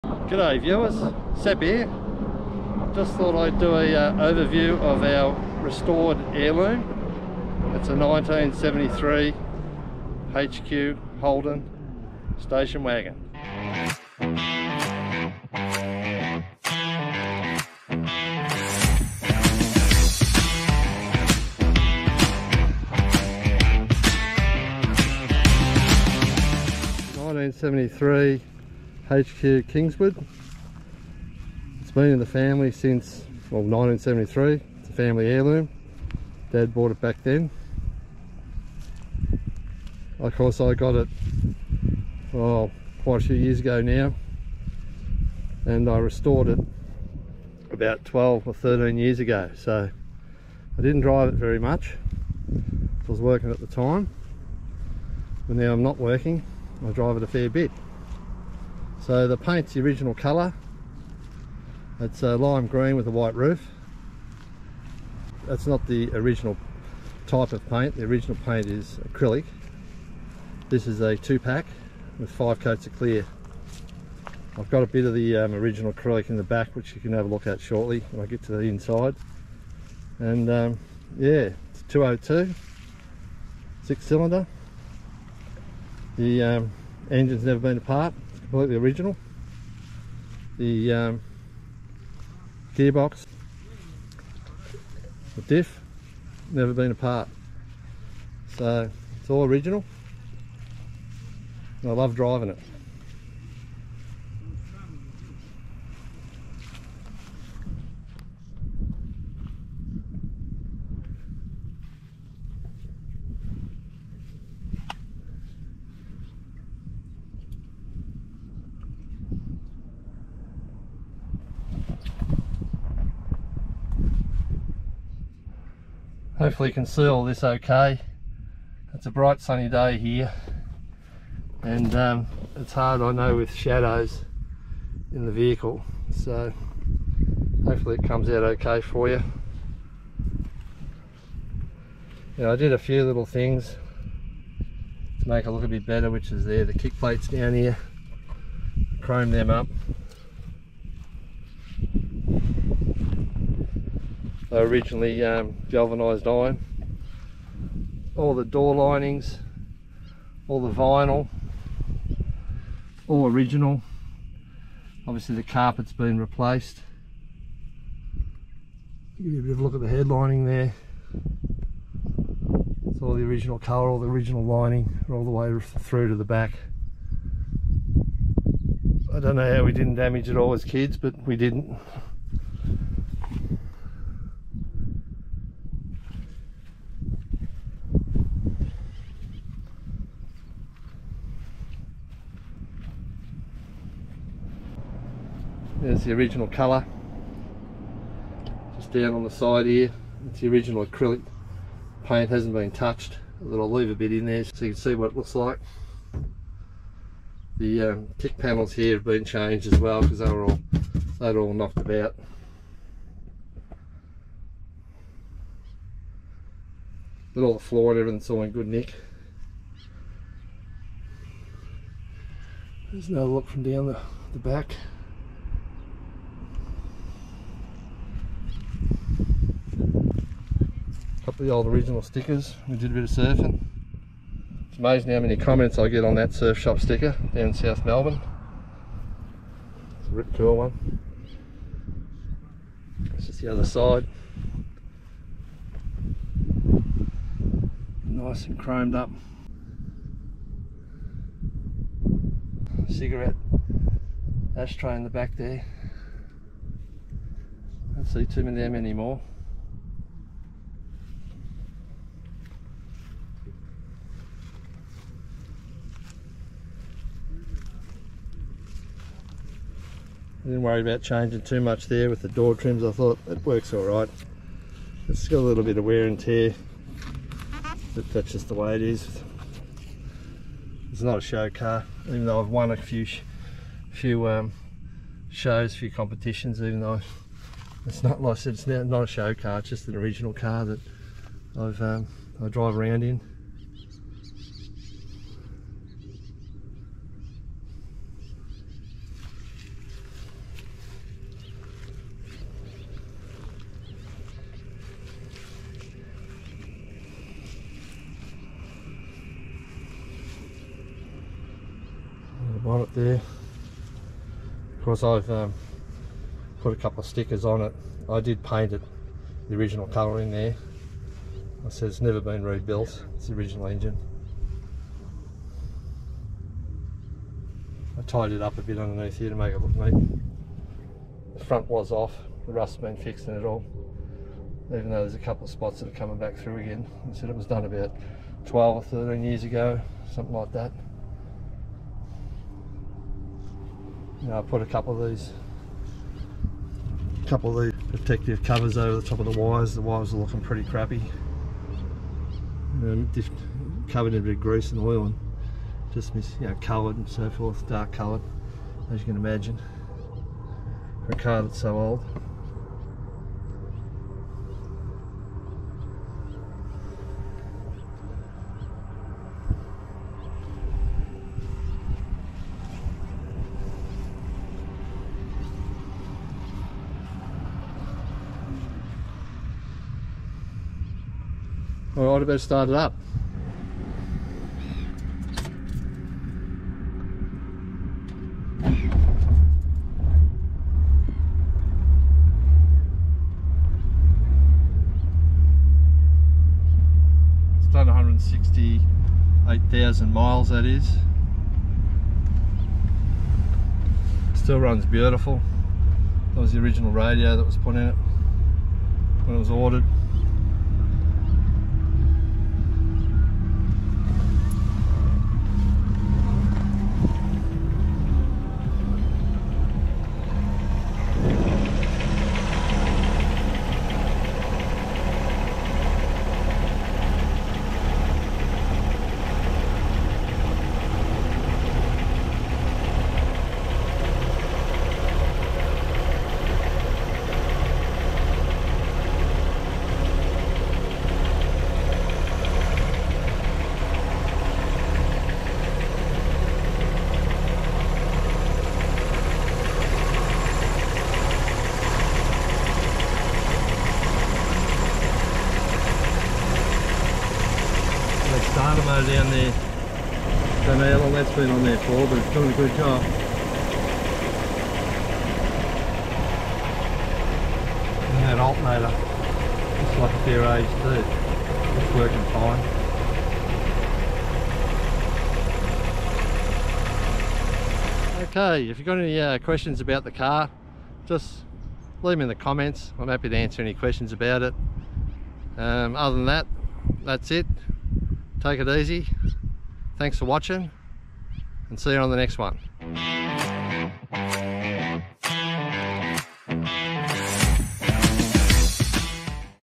HQ HOLDEN KINGSWOOD STATION WAGON RESTORATION OVERVIEW | SEPS ADVENTURES
The car was purchased brand new in 1973 by my father and has the original registration plates and purchase documentation. It was restored around 13 years ago by myself with the help of my now son-in-law who was a second year apprentice spray painter at the time. I stripped and assembled the car over a two year period and my son-in-law did the rust removal and painting. I have entered it in a couple of shows which it has won in its category. It’s not a show car but it’s in good original condition.